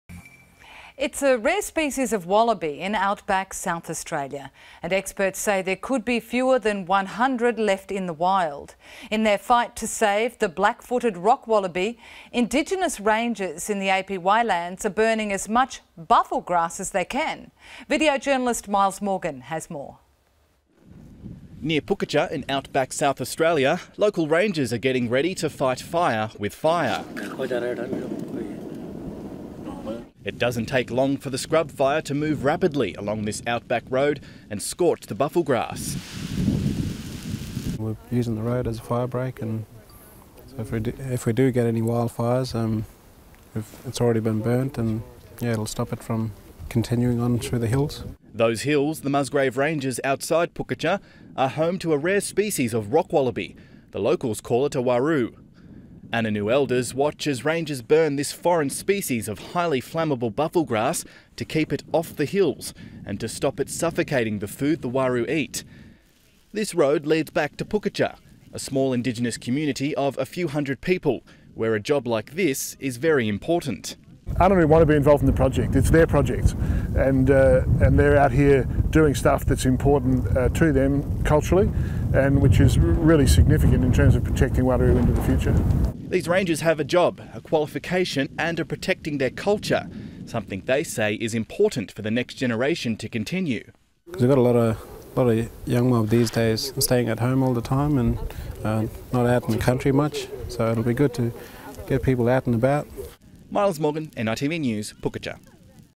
Bush Burning for the Black-footed Rock Wallaby
It's a rare species of wallaby in outback South Australia and experts say there could be fewer than one hundred left in the wild.